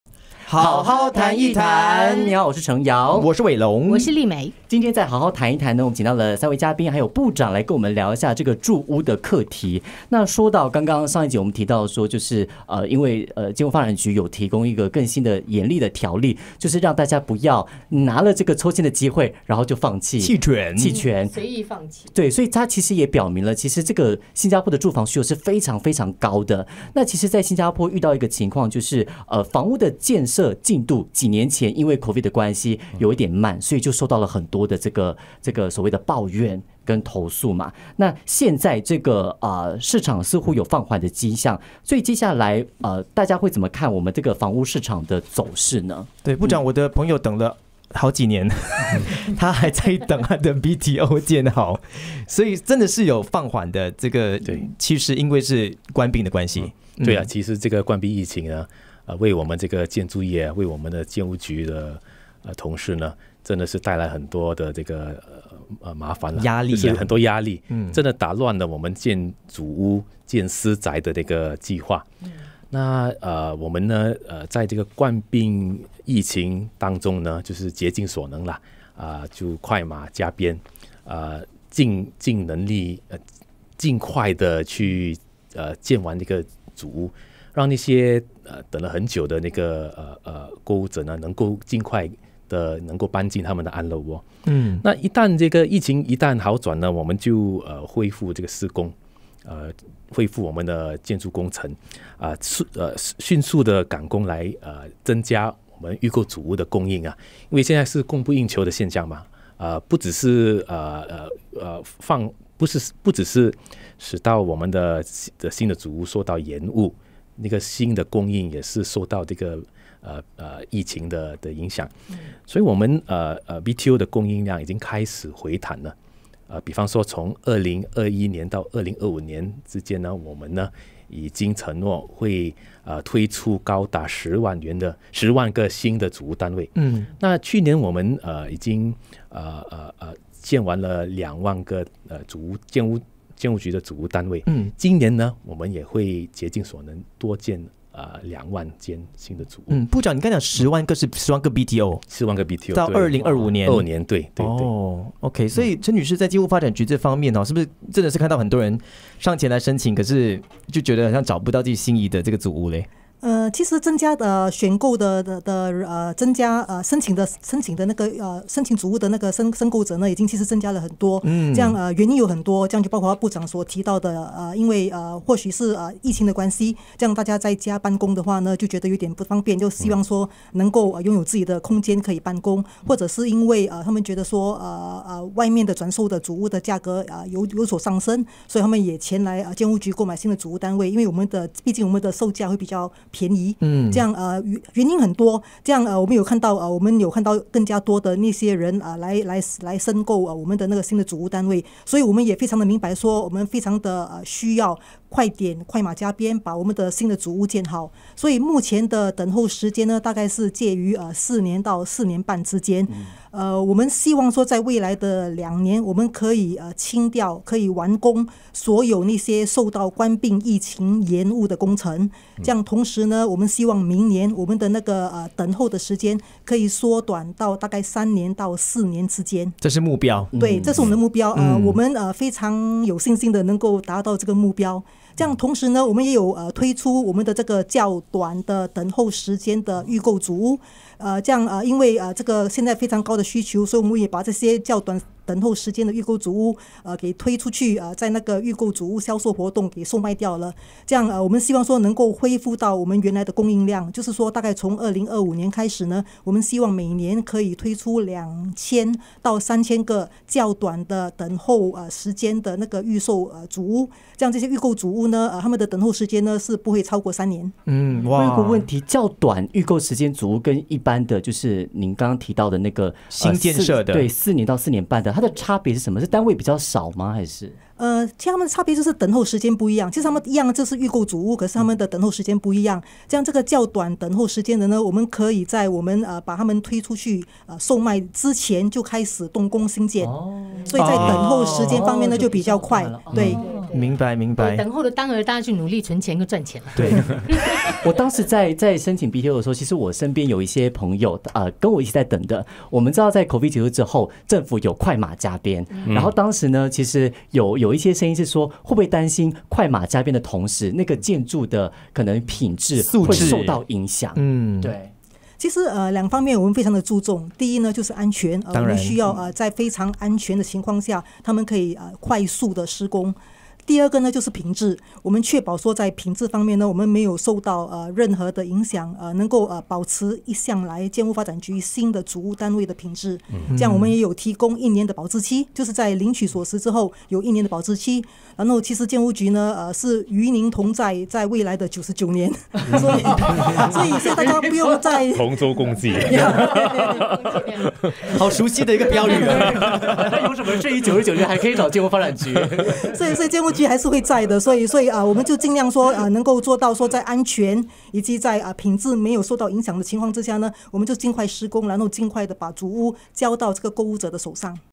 Turning the corner on BTO building programme
In this interview with UFM 100.3, Minister Desmond Lee shares updates on the Build-To-Order (BTO) building programme in Singapore.